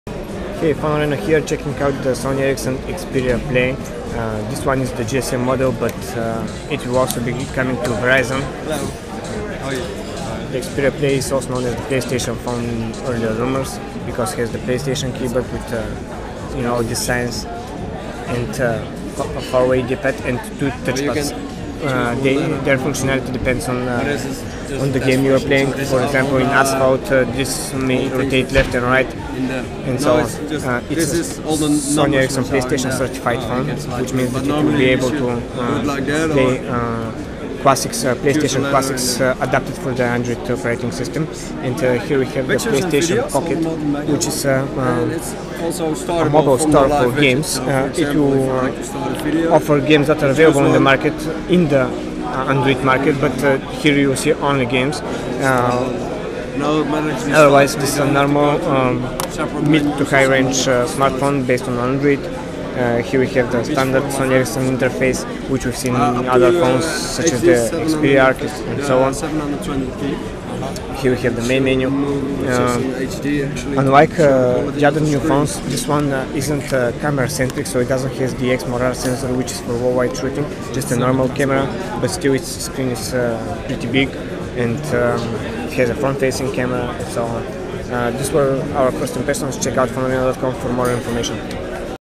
Sony Ericsson Xperia PLAY Hands-on
Live from MWC 2011: PhoneArena presents to you a hands-on of a prototype Sony Ericsson Xperia PLAY.